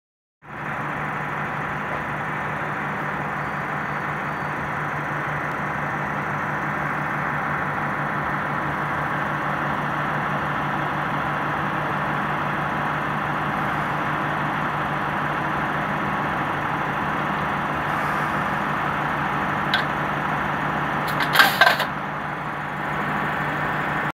CSR SDD-7 B960 acoplándose mediante el Sharfenberg a la formación de coches CSR Puzhen 21 en Retiro
El domingo 21 de agosto de 2022 a las 15.24